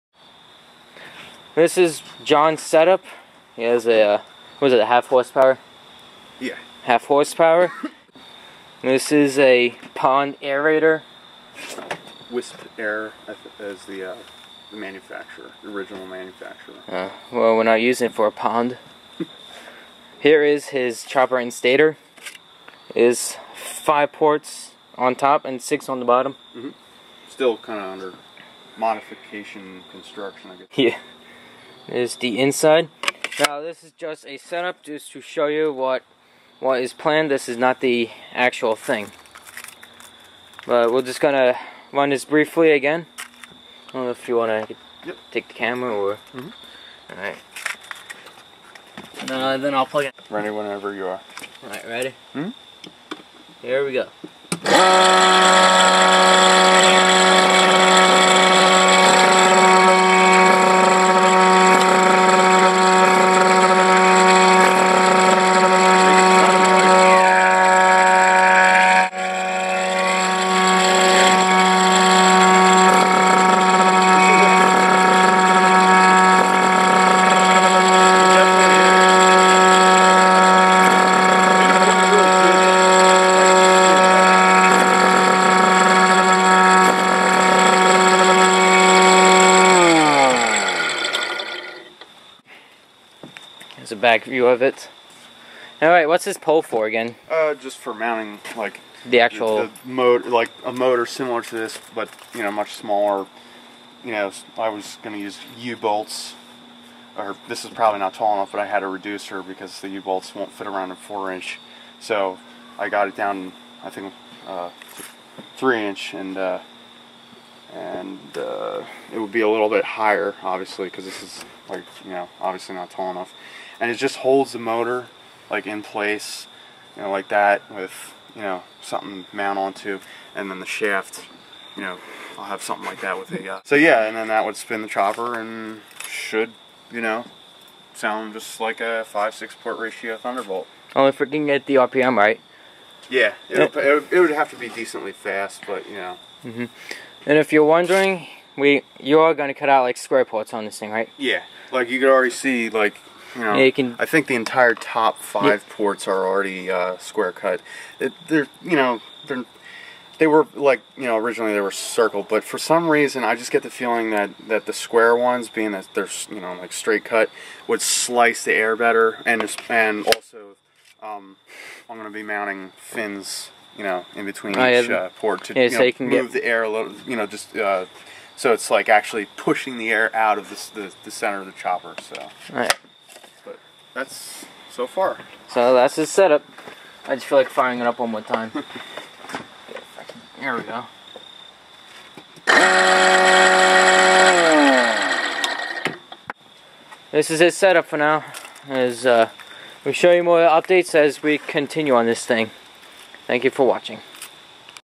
Homemade miniature Supercharged Siren video update
video update of the Homemade miniature Supercharged Siren. Nick came over and helped me put the parts back together to show the working demonstration. this is how it will.